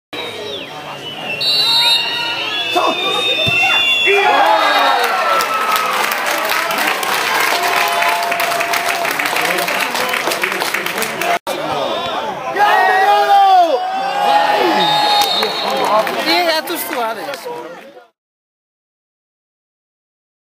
Feltrese-Treviso 2-0
Eccellenza Veneto girone B 2009/2010: l'A.S.D.Treviso 2009 chiude la sua avventura con una brutta sconfitta allo Zugni Tauro di Feltre: i padroni di casa vincono con un gol di Tormen e l'autorete di Borsato. Nel video i due gol.